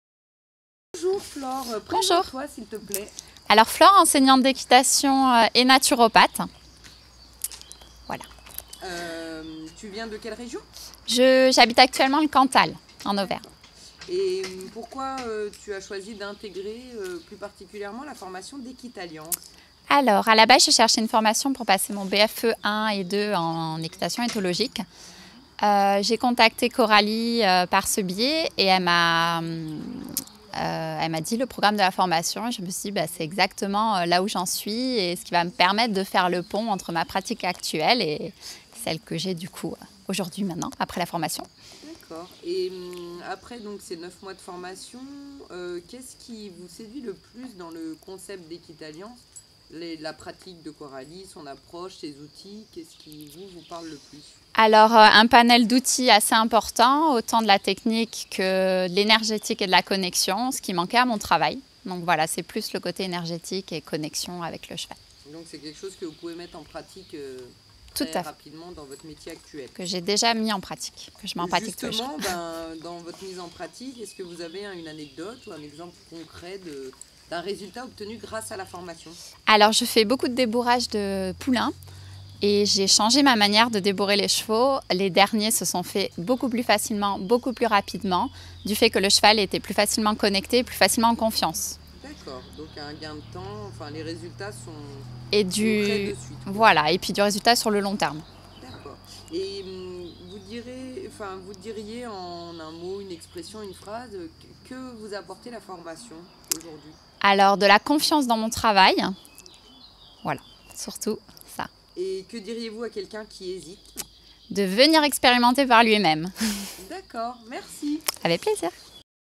Témoignage Flore Roux / Monitrice d'équitation BEES1°
Flore est aujourd'hui devenue formatrice pour la formation comportementaliste équin "module débourrage et manipulation du poulain" qui se déroule chez Nathalie Michel aux écuries de la Recorne en Bourgogne .
Vous pouvez la retrouver sur son site internet "www. equinaflore.com" ou sur sa page FB "Equinaflore Flore ROUX Equitation en conscience" !

Note : Flore est "comportementaliste équin nomade !" elle est aujourd'hui basée près de Tours (plus dans le cantal)